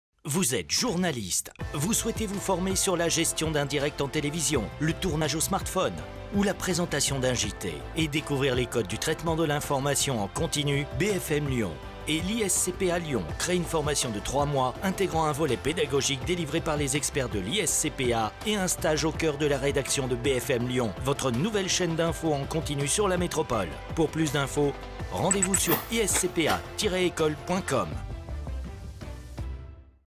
ISCPA LYON | BFM & ISCPA - Formation continue Journalisme
Vous êtes journaliste?
Vous souhaitez vous former sur la gestion d'un direct en télévision et découvrir les codes de l'information en continue?